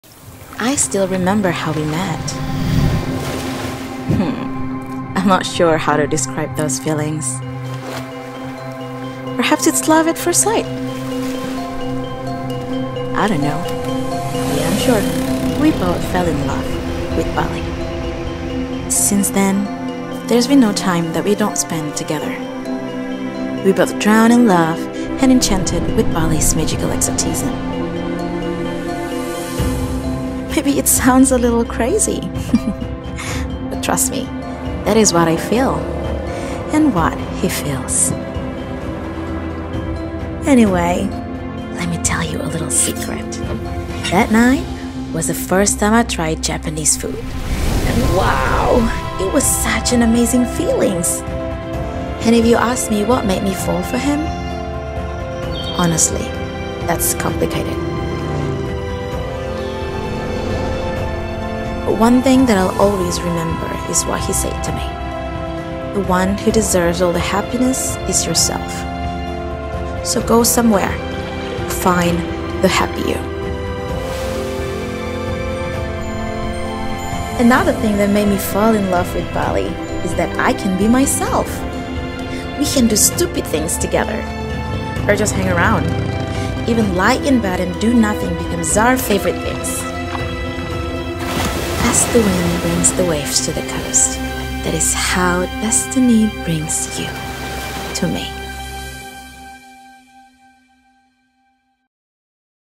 iNiViE Hospitality - THE JOURNEY OF LOVE | Campaign - Lewisual
The Journey of Love:
Capturing the beautiful bond between couples amidst the breathtaking landscapes of Bali with iNiViE Hospitality

Produced by Lewisual
Client INIVIE Hospitality